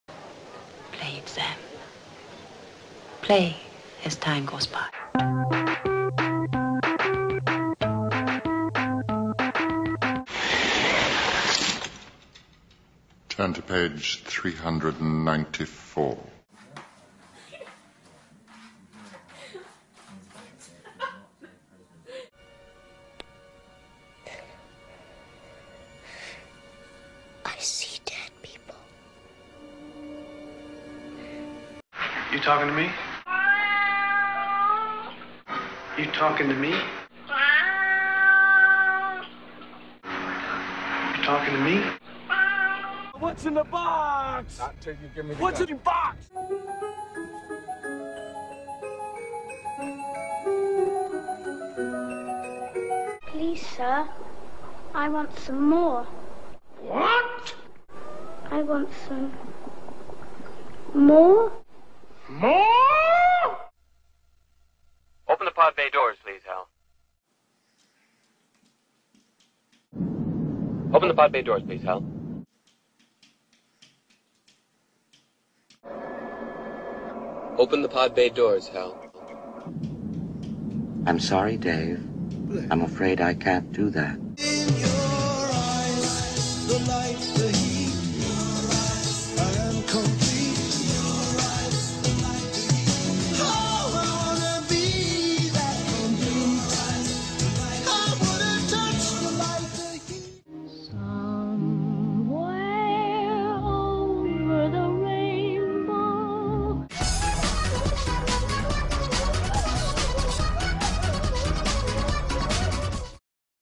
Movies Enhanced With YouTube Cats
Movie scenes enhanced with YouTube cats:
(edited to make it safe for all ages)